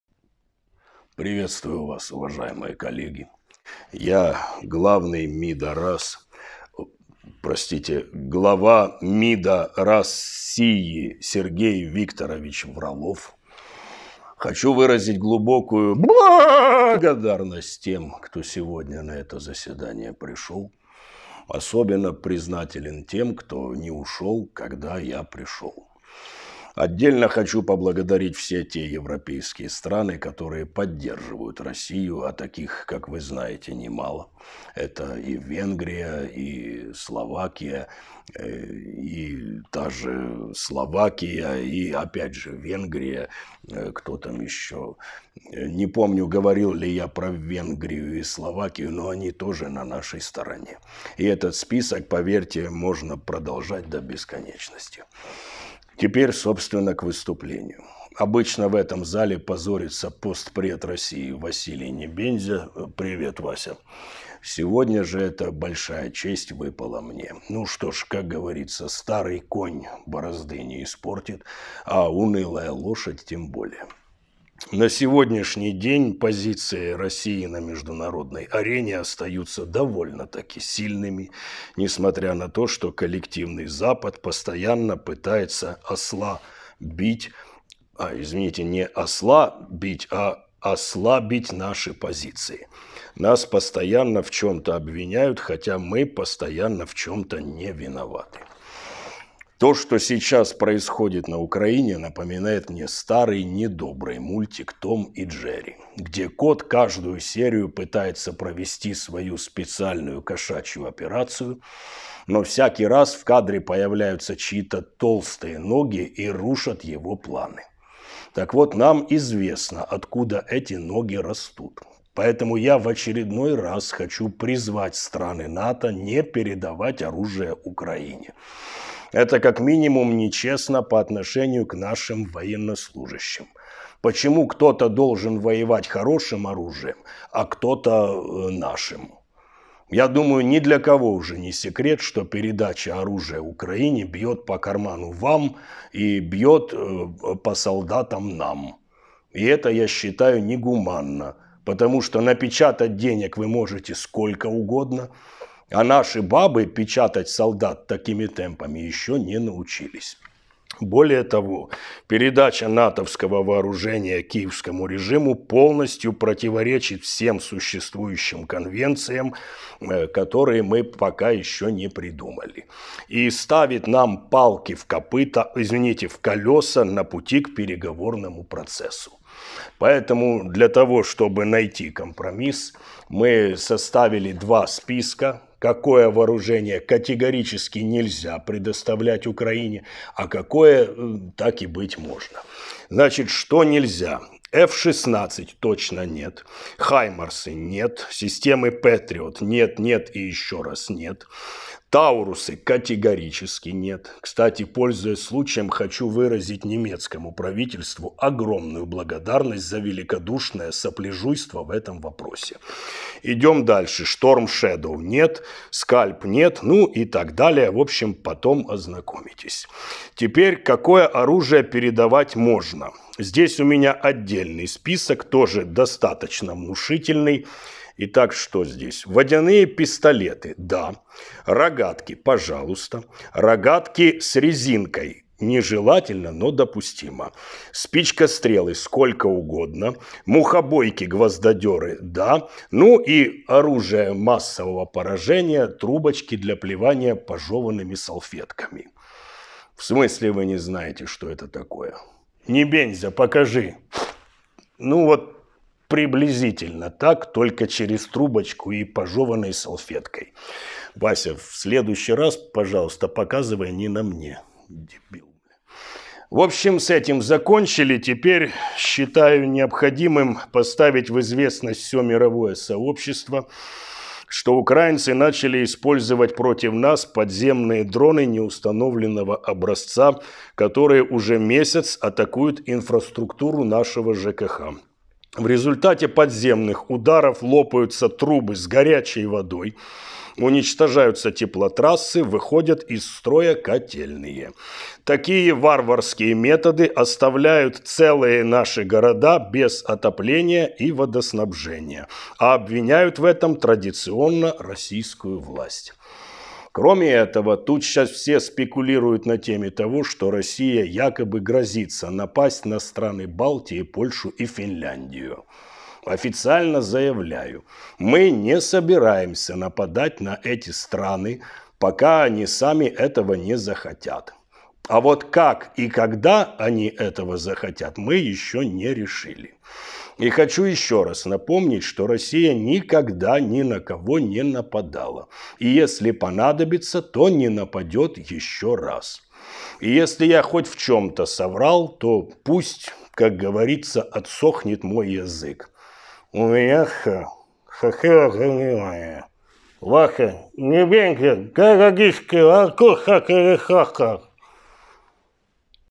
Такого бреда от ЛАВРОВА в ООН ещё не слышали 😁 [Пародия]
В последние годы россия активно использует такие платформы, как ОБСЕ и ООН для своих политических стендаперов, в лице Небензи и Лаврова. Их выступления уникальны, ведь они вызывают и смех, и отвращение одновременно. Сборники сочинений этих российских политических шутов, однозначно войдут в историю, как самые тупые, безумные и нелепые. А вершиной их карьеры, безусловно станет финальный стендап в ГААГЕ, где их уже давно ждут, вместе с их главным бункерным режиссёром-постановщиком. В этой пародии вашему вниманию предоставляется новая порция феноменального кремлёвского бреда.
Сміємось - отже не здаємось ✊

Дякую співавторам за допомогу 👍